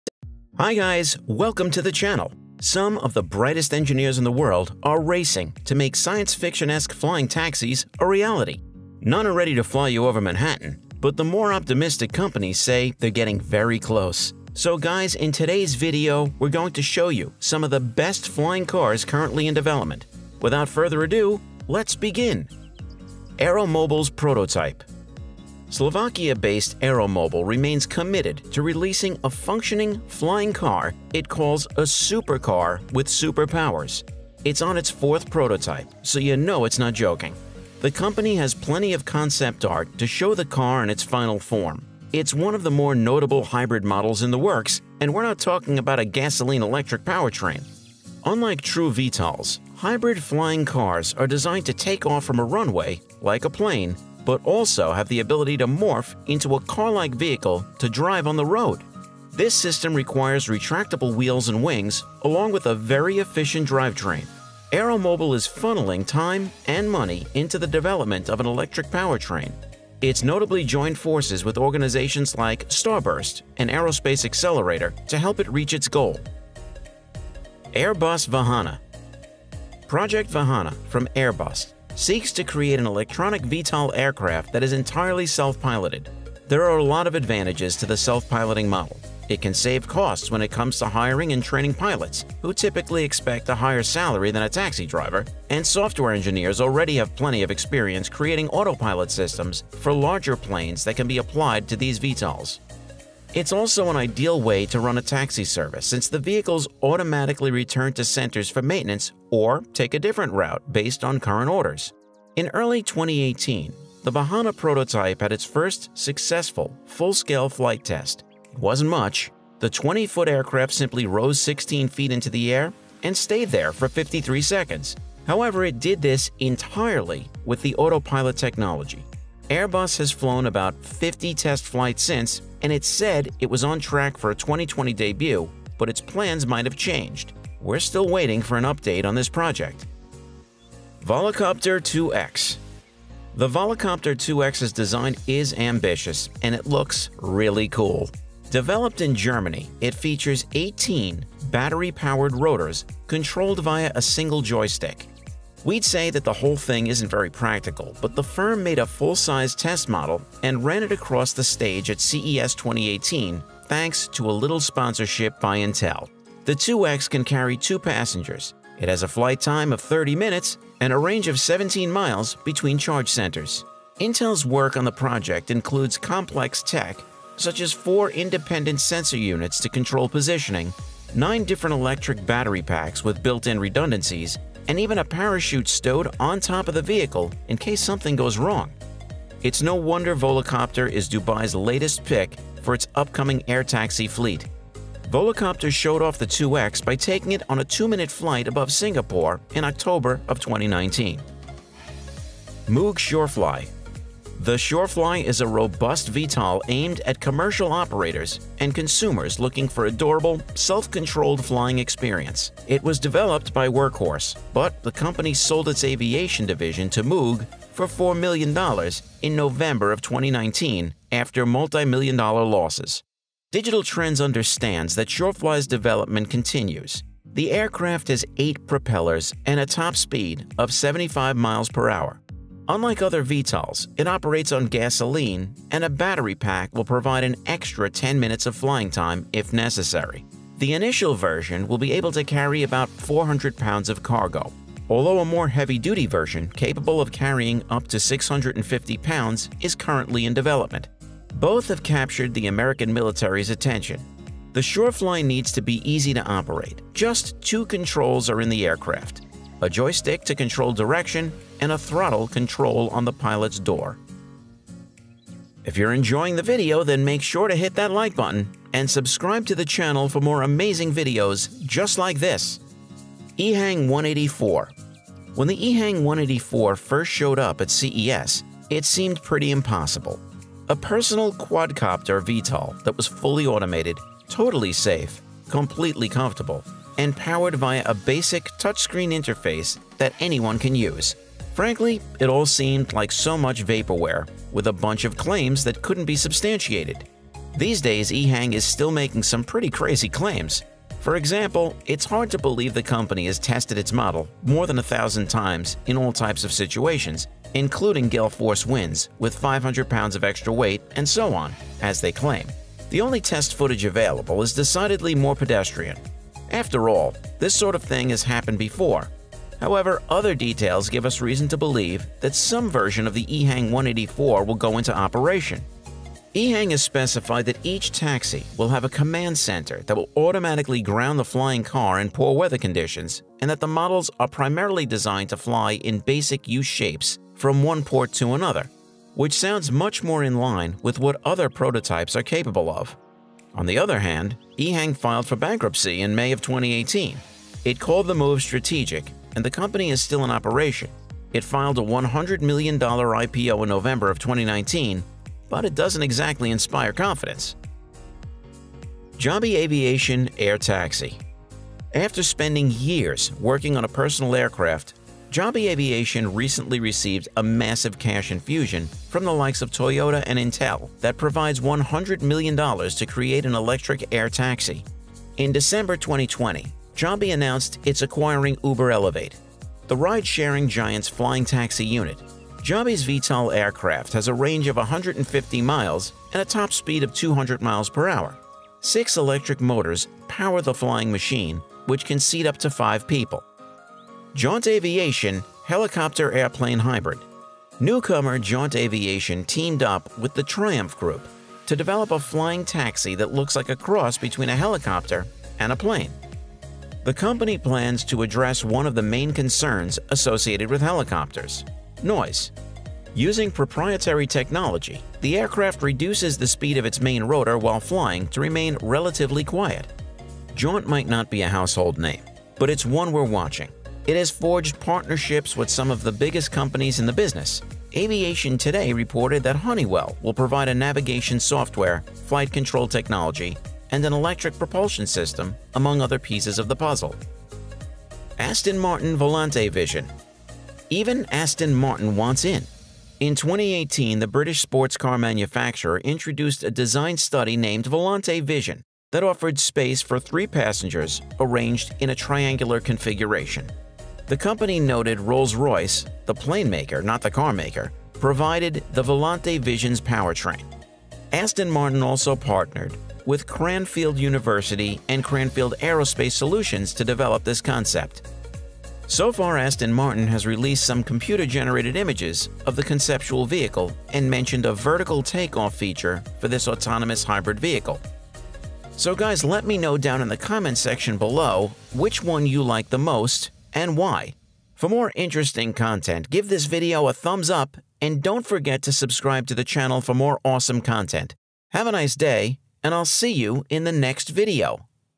IT HAPPEND, Flying Cars Are Here!
Flying Cars That Actually Fly!

For as long as we've been driving cars, people have been trying to figure out how to make them fly.
That dream is a lot closer to reality than we think.
Some of the brightest engineers in the world are racing to make science fiction-Esque flyingtaxis a reality. None are ready to fly you over Manhattan, but the more optimistic companies say they’re getting very close.

About Facts About Tech
🎥 Videos about Elon Musk, Tech And Gadgets!
🎨 Written, voiced and produced by Facts About Tech